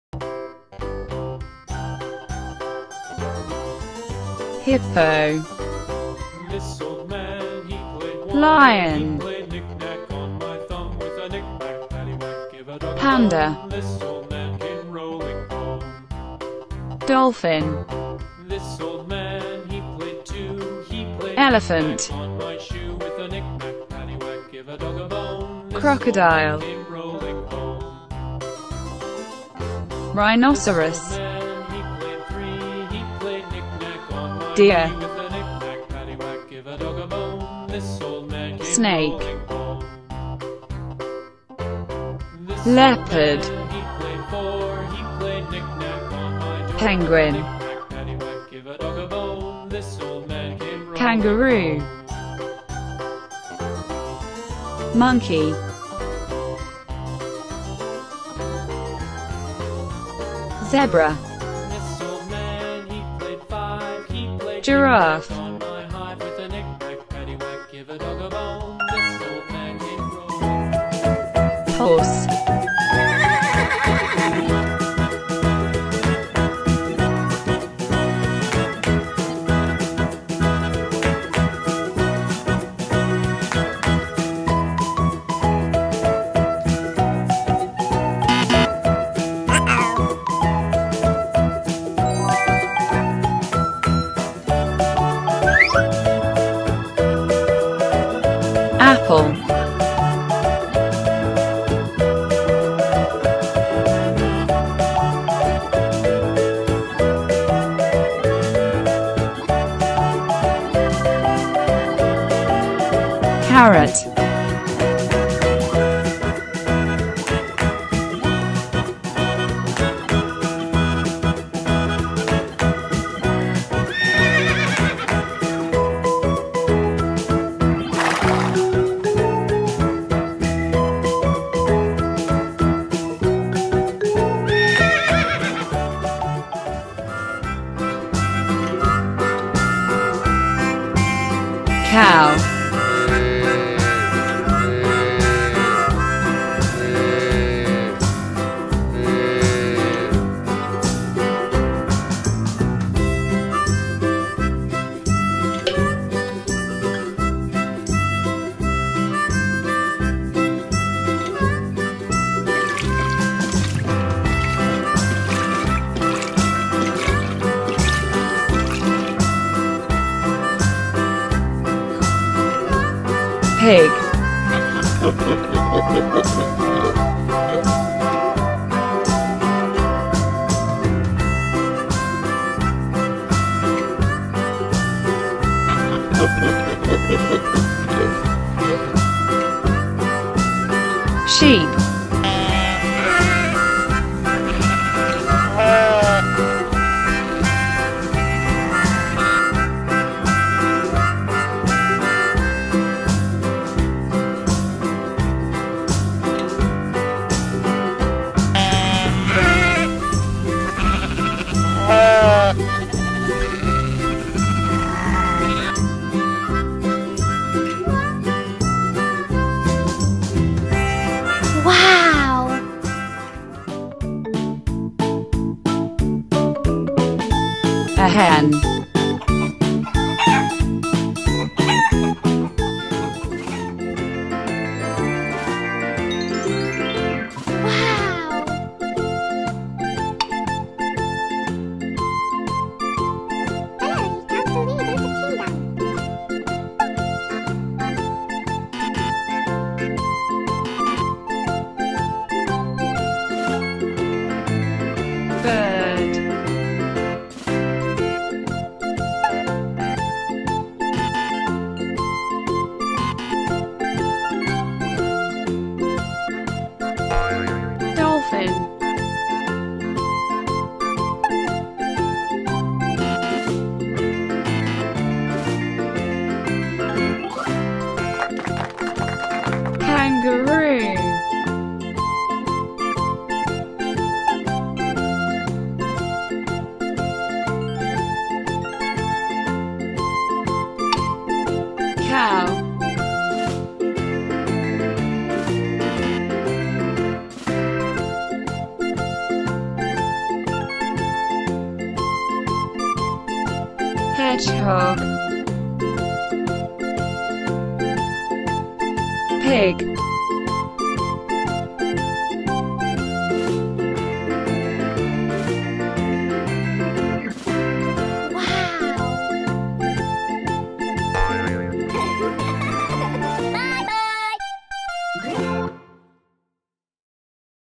Learn Animals With Toy Blocks | Pretend To Play Indoor Playground Farm | Animal Shapes Puzzle
A fascinating toy for children, in which it is necessary to guess an animal according to its shapes! Farm Animals For Kids, Children And Toddlers!